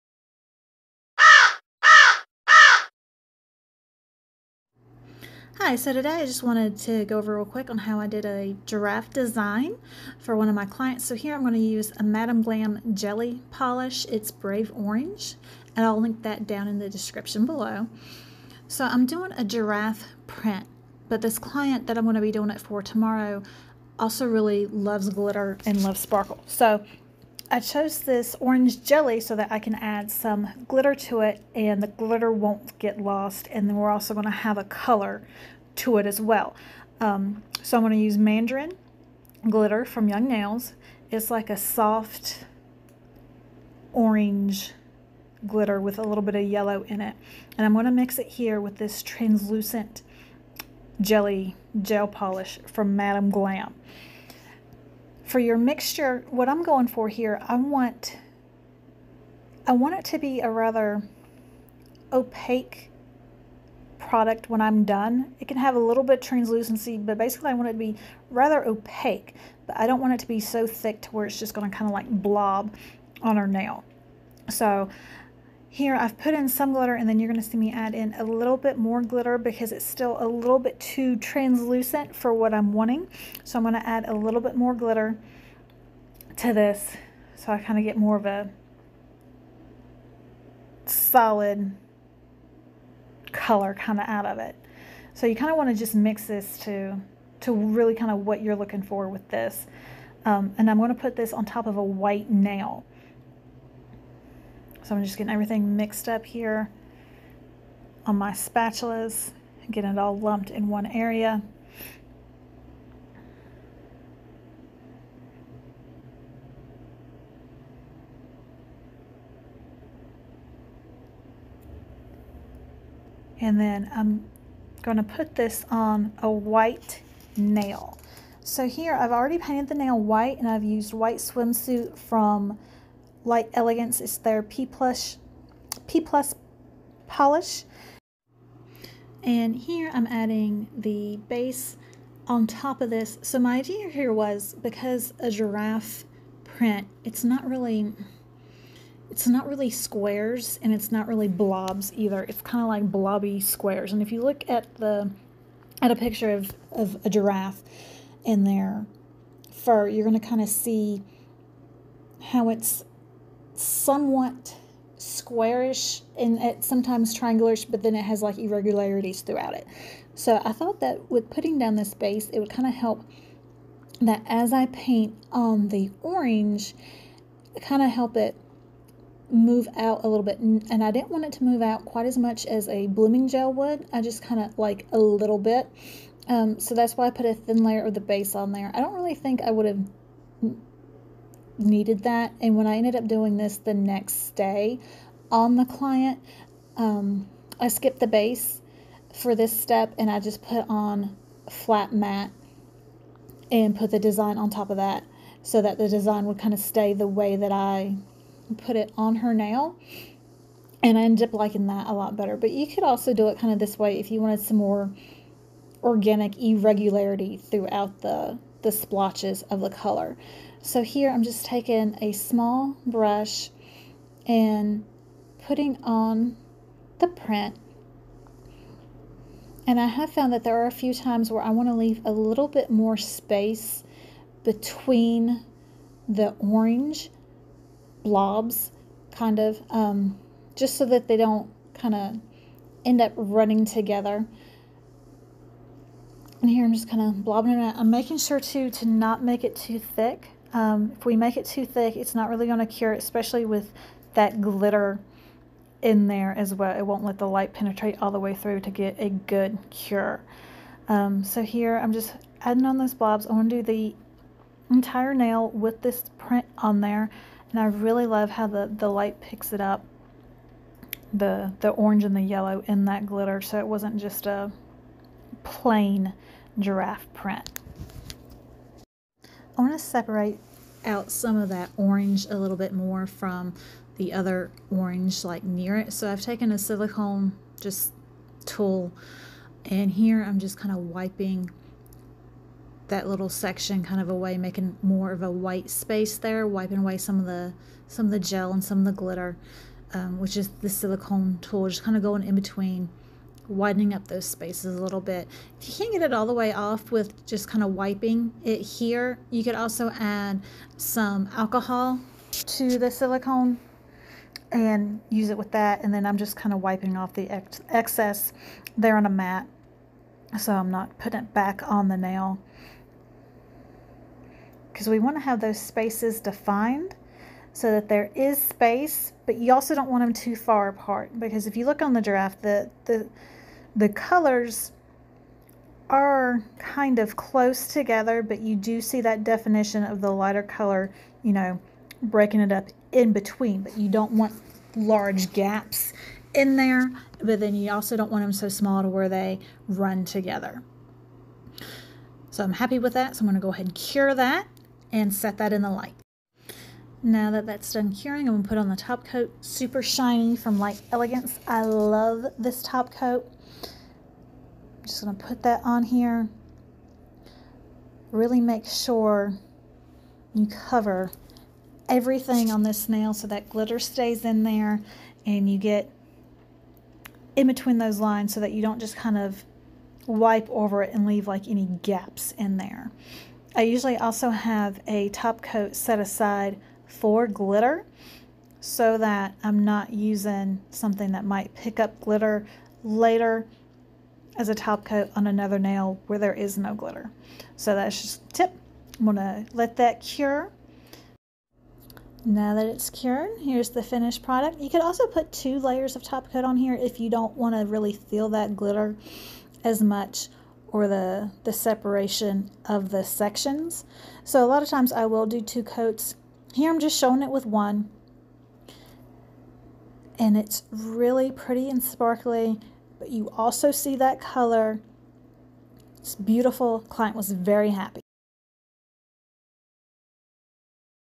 Glitter Giraffe Print Nail Tutorial
A tutorial on adding glitter to create a sparkly giraffe print on nails. Products used:

Follow The Crow's Claw on Instagram and TikToc @thecrowsclawnails